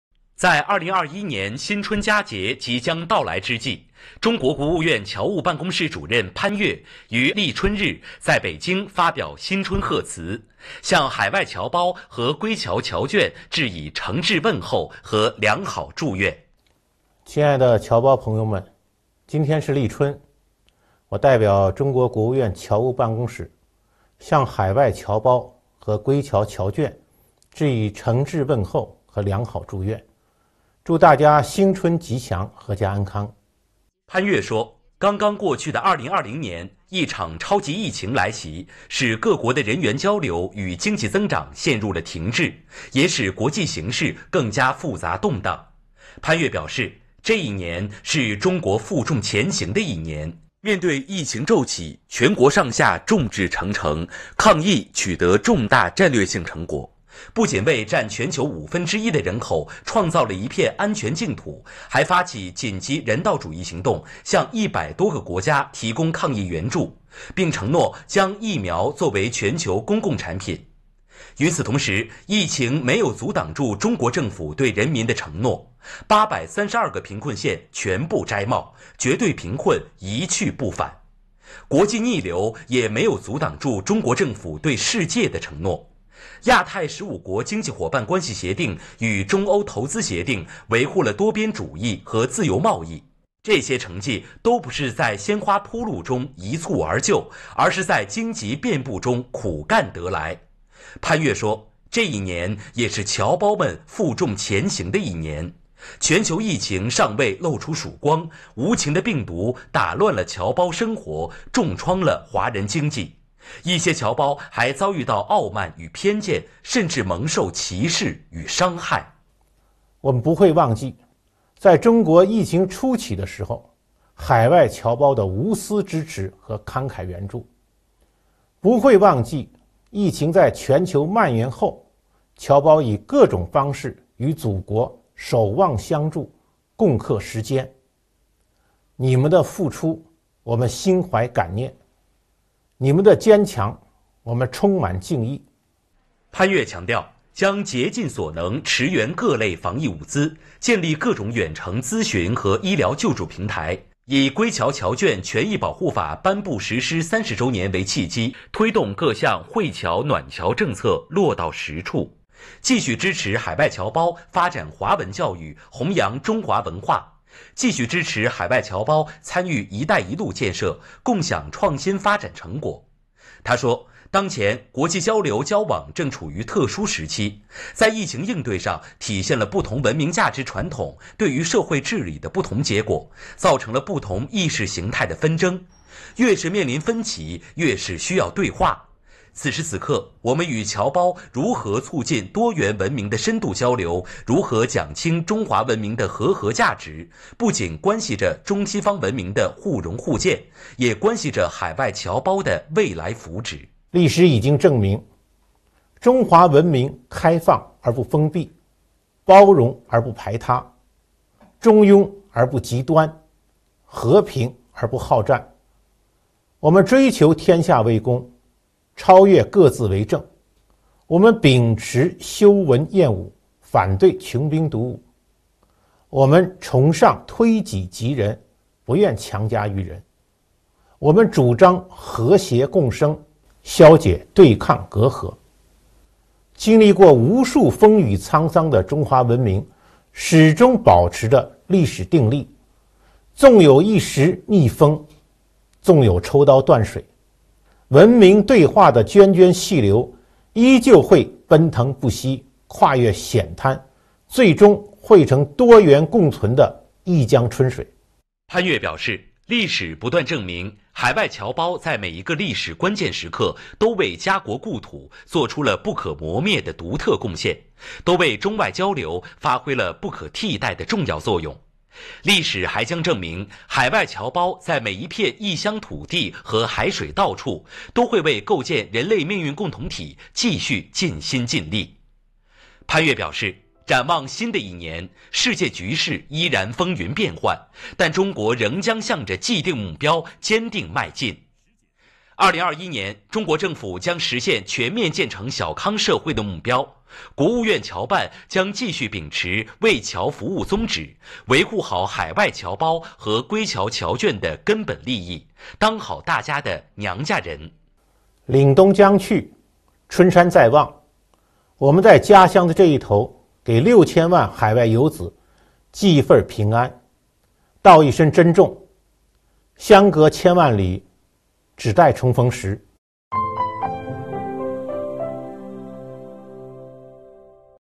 中国国务院侨办主任潘岳发表新春贺辞
在2021年新春佳节即将到来之际，中国国务院侨务办公室主任潘岳于立春日在北京发表新春贺辞，向海外侨胞和归侨侨眷致以诚挚问候和良好祝愿。
         潘岳表示，这一年，是中国负重前行的一年。面对疫情骤起，全国上下众志成城，抗疫取得重大战略性成果，不仅为占全球五分之一的人口创造了一片安全净土，还发起紧急人道主义行动，向100多个国家提供抗疫援助，并承诺将疫苗作为全球公共产品。与此同时，疫情没有阻挡住中国政府对人民的承诺，832个贫困县全部“摘帽”，绝对贫困一去不返。国际逆流也没有阻挡住中国政府对世界的承诺，亚太15国经济伙伴关系协定与中欧投资协定，维护了多边主义和自由贸易。这些成绩，都不是在鲜花铺路中一蹴而就，而是在荆棘遍布中苦干得来。
        潘岳说，这一年，也是侨胞们负重前行的一年。全球疫情尚未露出曙光，无情的病毒打乱了侨胞生活，重创了华人经济；一些侨胞还遭遇到傲慢与偏见，甚至蒙受歧视与伤害。
        他说，当前，国际交流交往正处于特殊时期，在疫情应对上，体现了不同文明价值传统对于社会治理的不同结果，造成了不同意识形态的纷争。越是面临分歧，越是需要对话。此时此刻，我们与侨胞如何促进多元文明的深度交流，如何讲清中华文明的和合价值，不仅关系着中西方文明的互融互鉴，也关系着海外侨胞的未来福祉。
        潘岳表示，历史不断证明，海外侨胞在每一个历史关键时刻，都为家国故土作出了不可磨灭的独特贡献，都为中外交流发挥了不可替代的重要作用。历史还将证明，海外侨胞在每一片异乡土地和海水到处，都会为构建人类命运共同体继续尽心尽力。
        潘岳表示，展望新的一年，世界局势依然风云变幻，但中国仍将向着既定目标坚定迈进。2021年，中国政府将实现全面建成小康社会的目标。国务院侨办将继续秉持为侨服务宗旨，维护好海外侨胞和归侨侨眷的根本利益，当好大家的娘家人。